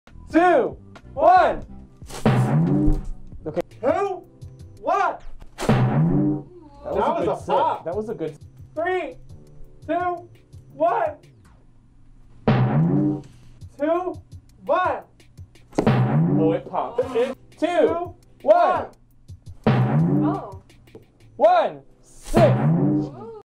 Strobes and Gale FrostBane sitting with Cartoon Sound Effects
Here is a random clip of my two favorite furries testing out an egg sitter invention
This video is just a for a harmless joke 👍 enjoy y’all